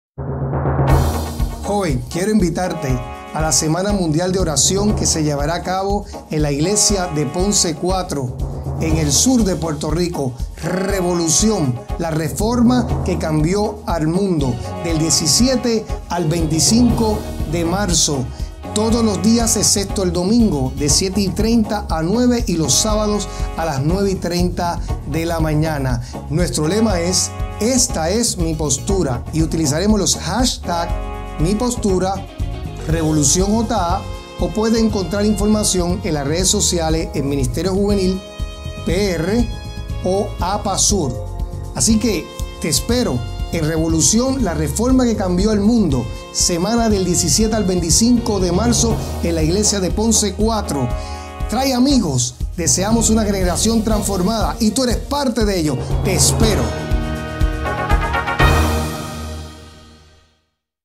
Revolución: La Reforma que cambió al mundo ¡Esta es mi Postura!
Semana Mundial de Oración Juvenil 2017
Revolución: La Reforma que cambió al mundo
Evento de la Juventud de la Asociación Adventista del Sur
En Ponce IV